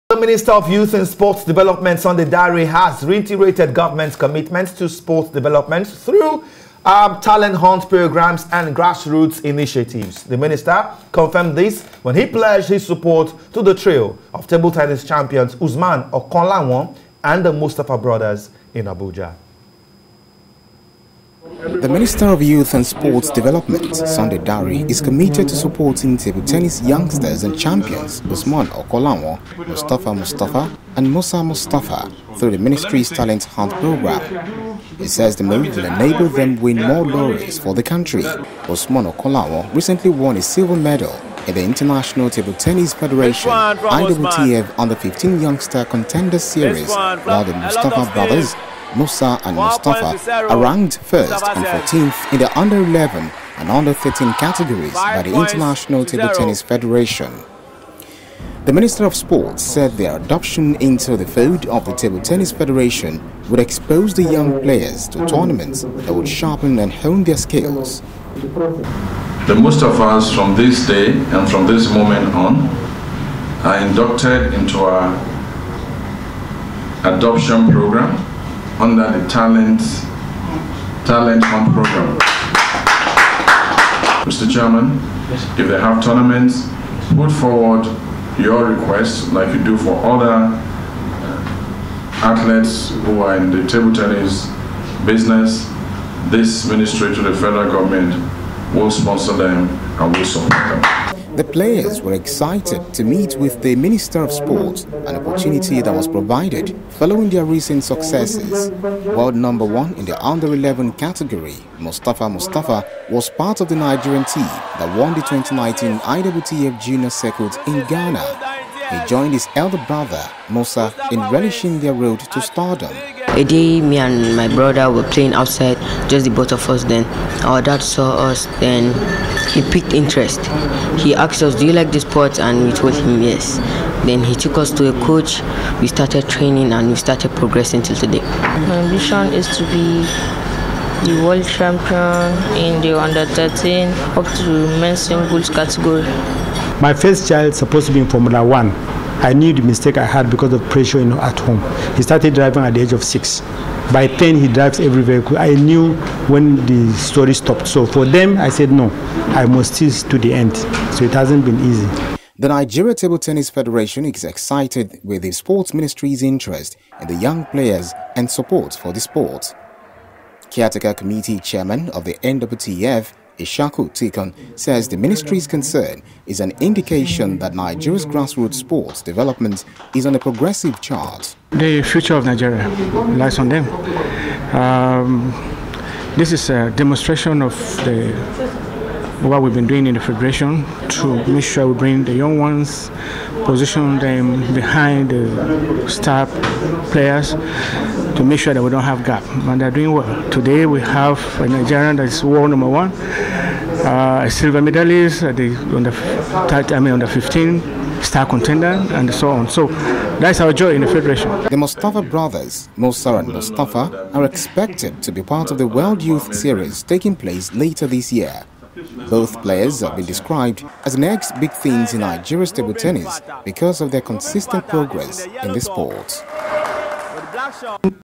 Talent Hunt: Sports Minister Adopts Table Tennis Prodigies, Okanlawon And Mustapha Brothers
The Minister of Youth and Sports Development, Sunday, has adopted table tennis prodigies, Okanlawon and Mustapha brothers as parts of his talent hunt programme.